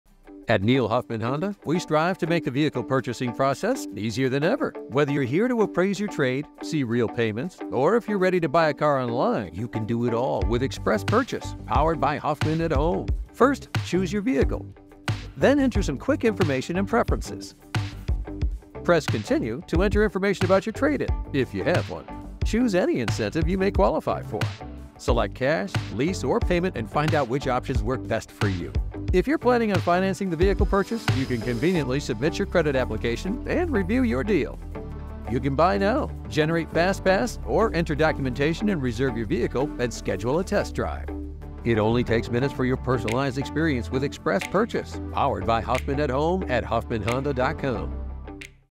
Express Purchase at Neil Huffman Honda | Clarksville, IN
A fast, easy, & hassle-free car-buying experience, all from the comfort of your home!